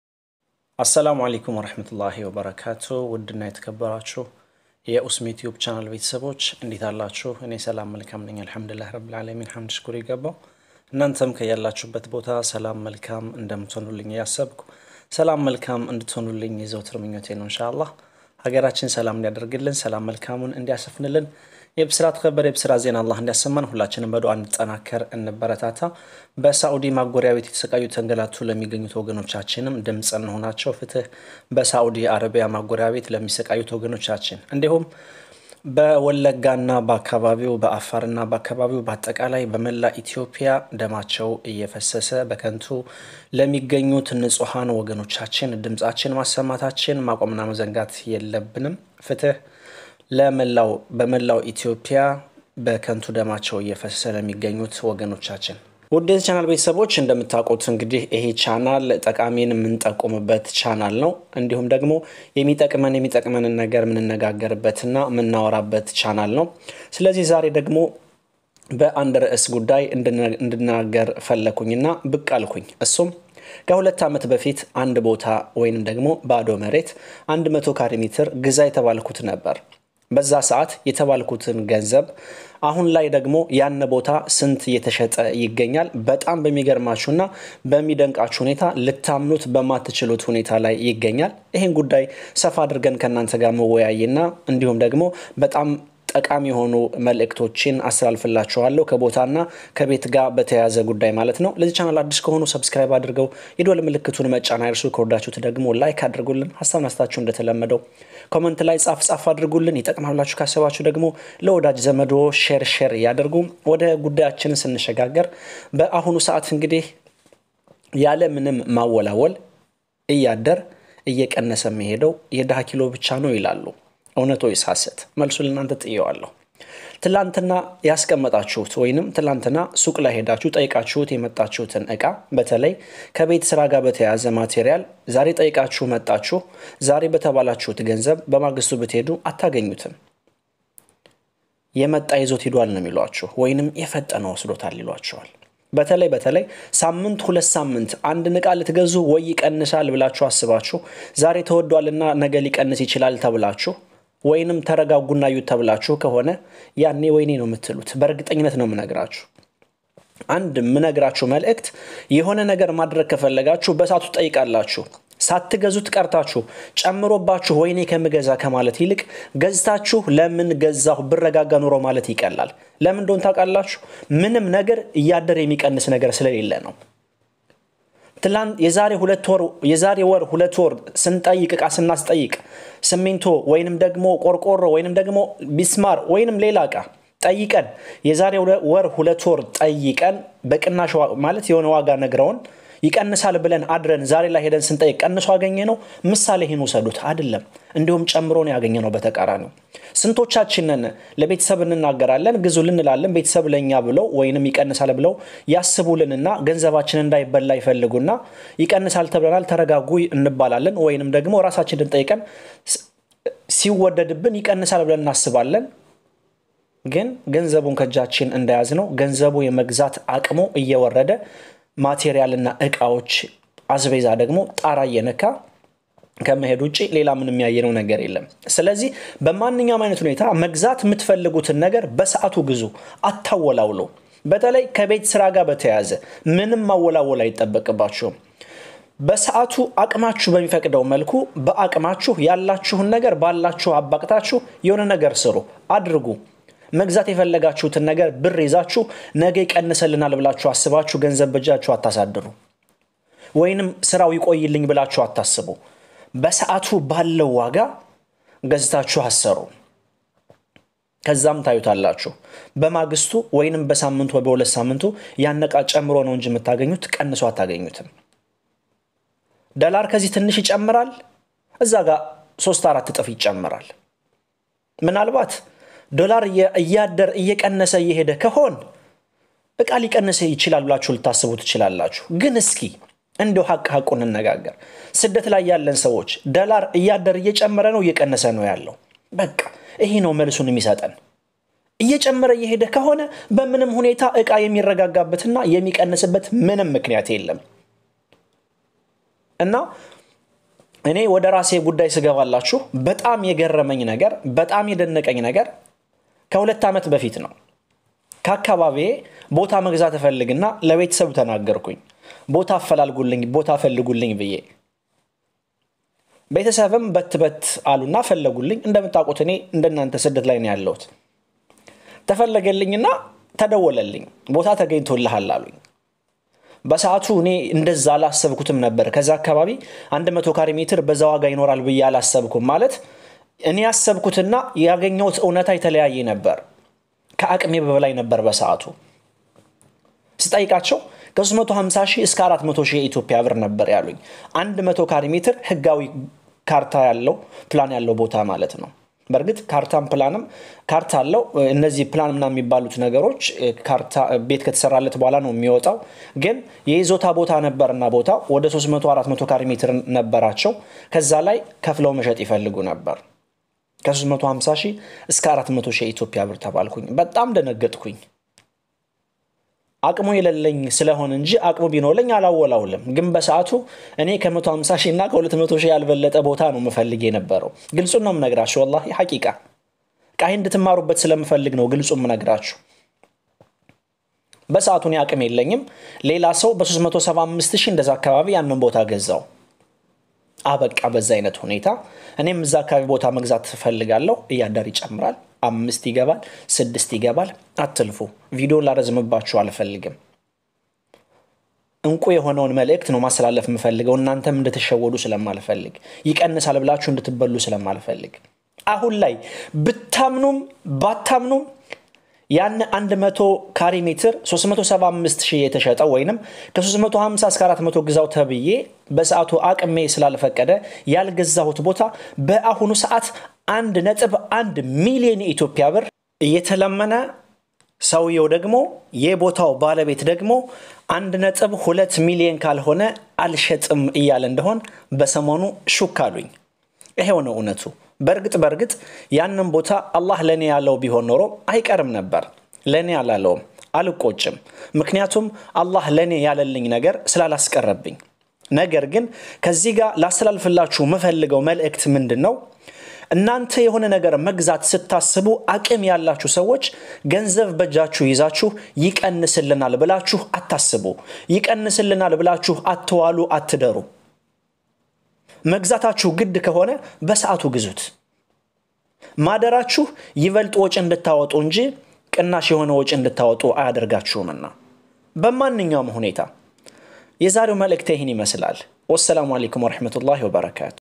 ጉድ እኮነው ህጋዊ 100ካሬ መሬት በቅናሽ ግዛው የተባልኩት ቦታ ዛሬ ሚሸጥበትን ዋጋ ስሰማ ፌንት ልወጣ!እንዴ እንዴ😂😱 ለማመን ያዳግታል ከኡስሚ tube
መደመጥ ያለበት ወሳኝ መረጃ!